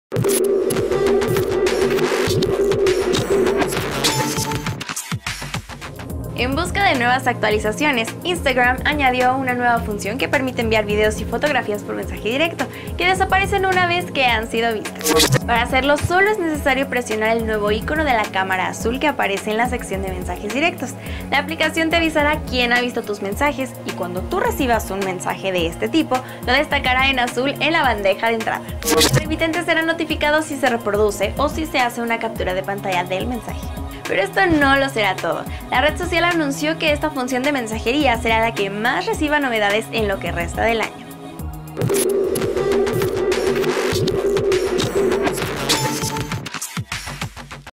Instagram Direct- Hi-Tech
En busca de nuevas actualizaciones, Instagram añadió una nueva función que permite enviar videos y fotografías por mensaje directo.